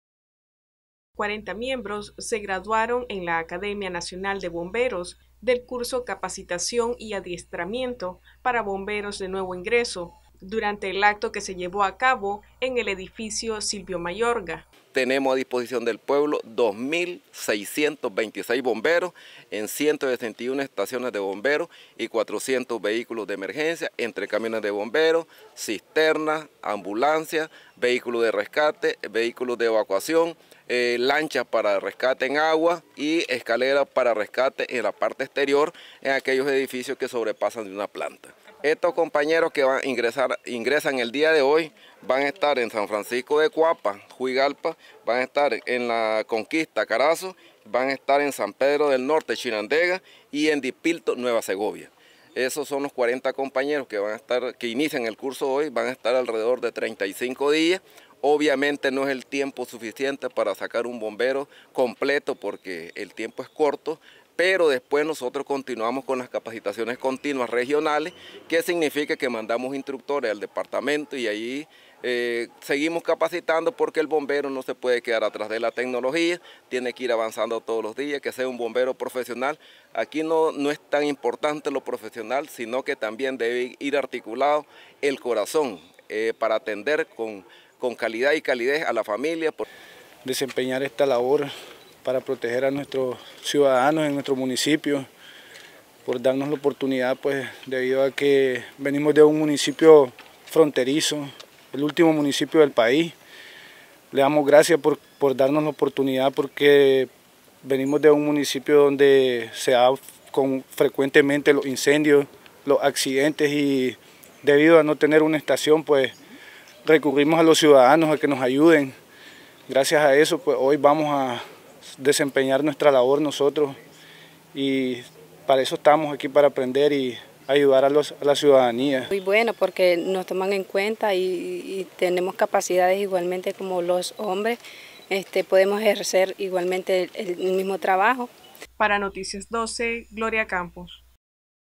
Noticias12Nicaragua 40 nuevos bomberos participan del Curso "Capacitación y entrenamiento"
40 nuevos bomberos participan del Curso "Capacitación y entrenamiento"